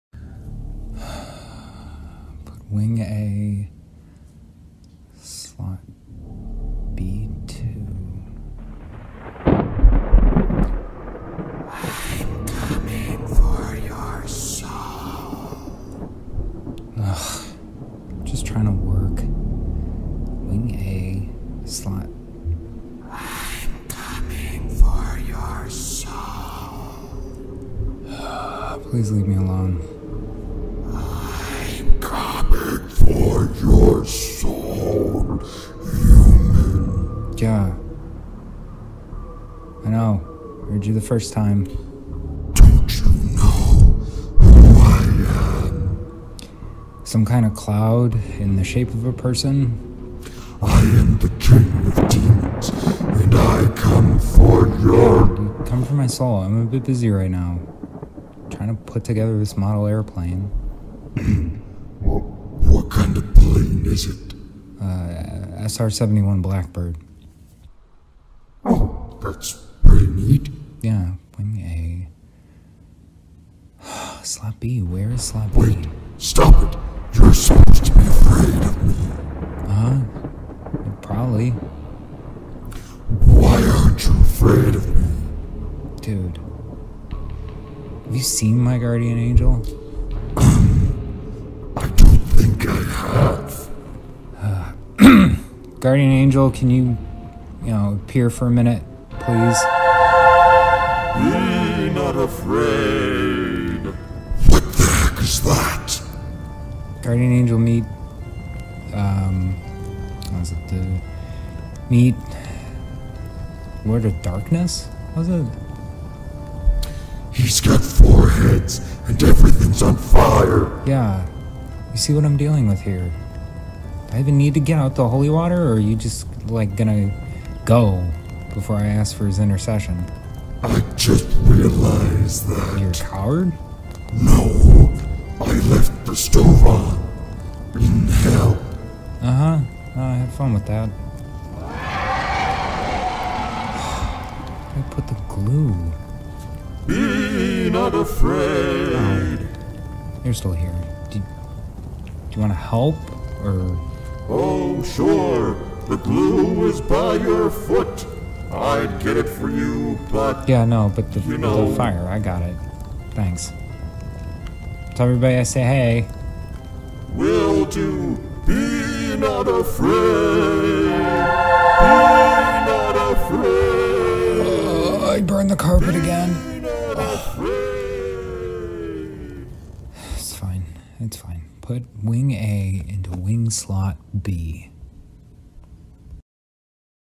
Satan Vs Guardian Angel
A man is attacked by Satan. He is unimpressed.